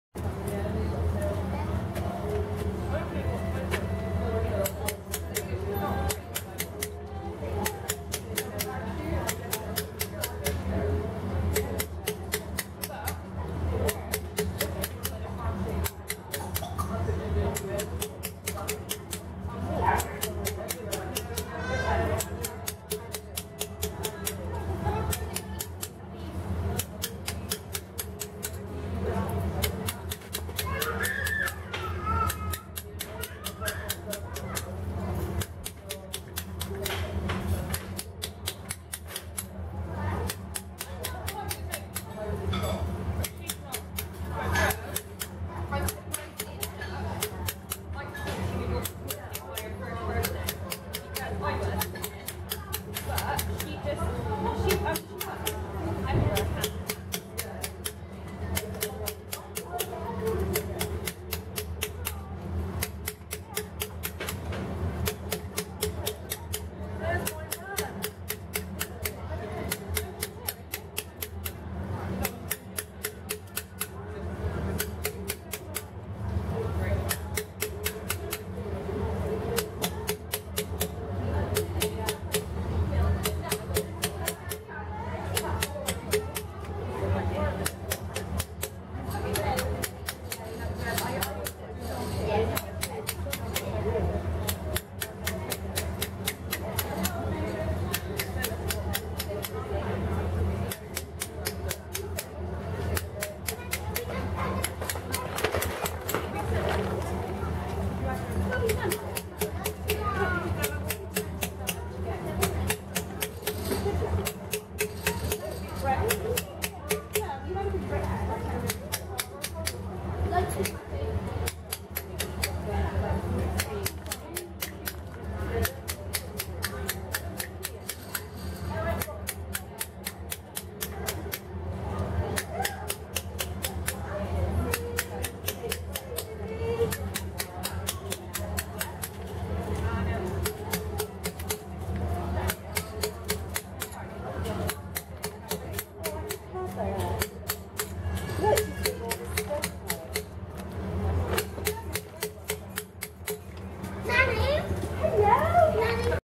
Carving my WWII soldier
Here I am carving the WWII soldier for the new war memorial for Mitcheldean and Abenhall.